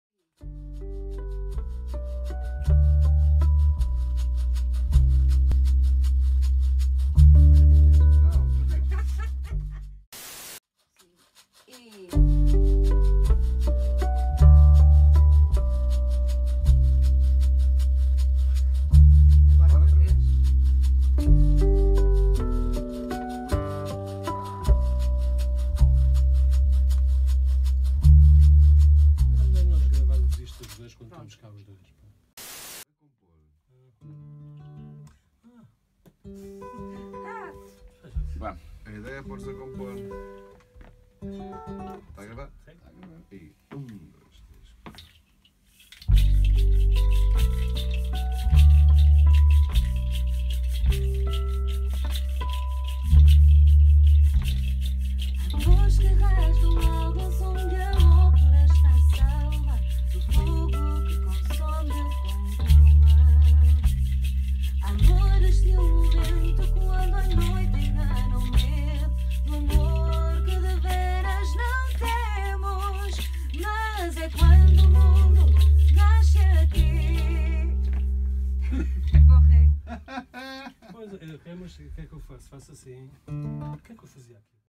FLOR DE LIS studio sessions 2020 [COMPOSITION // 08 February]
Sessões de estúdio para o álbum de Flor de Lis a sair em 2020: composição de frases de guitarra.